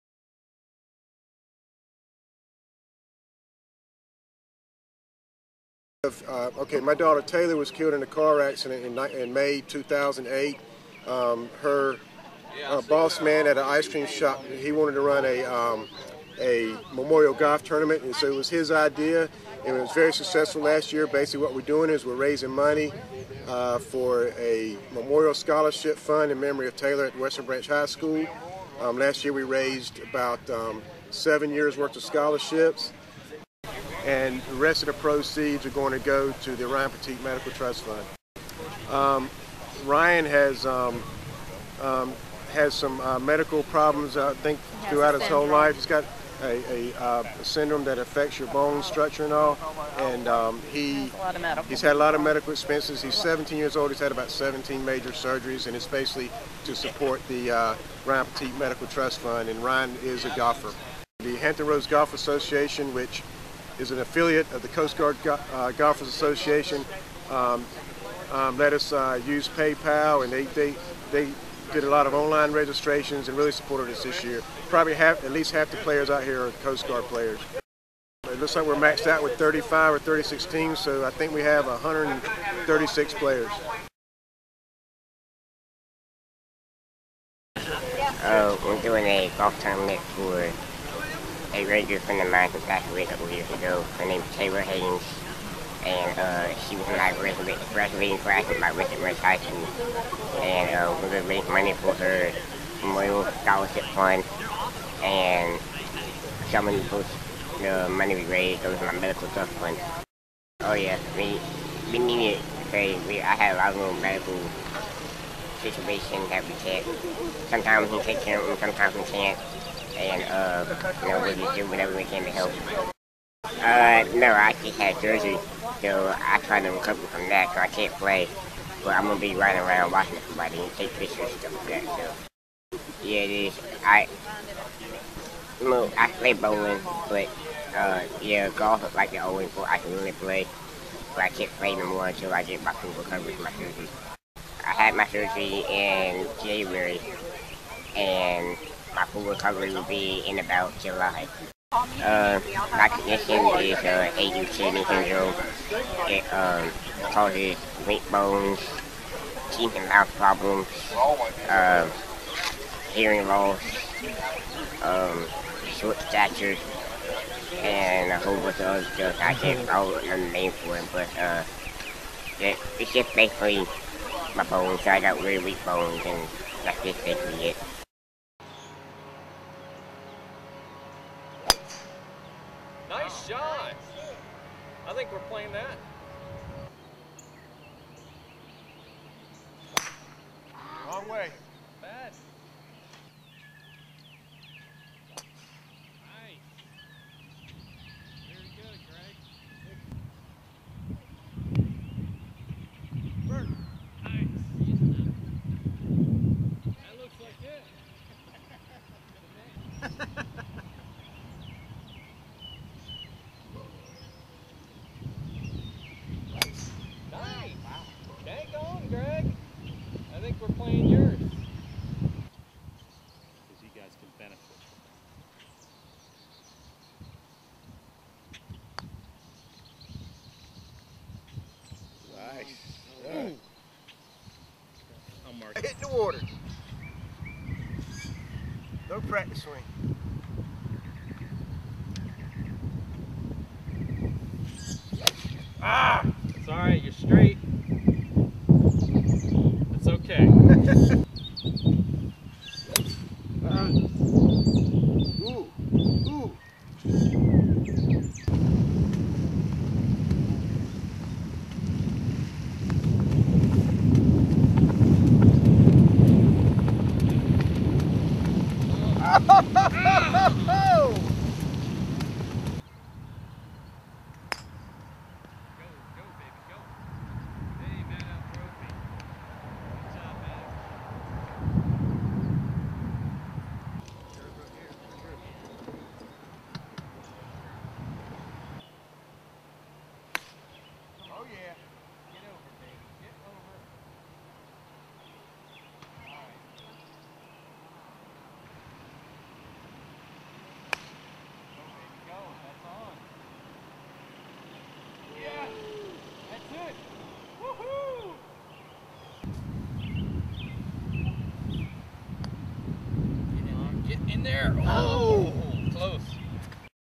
2nd annual Taylor Hayes Memorial Golf Tournament
PORTSMOUTH, Va. - A 5th District Coast Guard member in the Hampton Roads area held the 2nd annual Taylor Hayes Memorial Golf Tournament at the Sleepy Hole Golf Course in Suffolk, Va., in memory of his daughter on Apr. 9, 2010. Lt. Greg Hayes' 16-year-old daughter was killed in an auto accident in May 2008 and he began a memorial tournament to promote seatbelt safety and it raises money for a Western Branch High School Scholarship.

U.S. Coast Guard video by Petty Officer 2nd Class Andrew Kendrick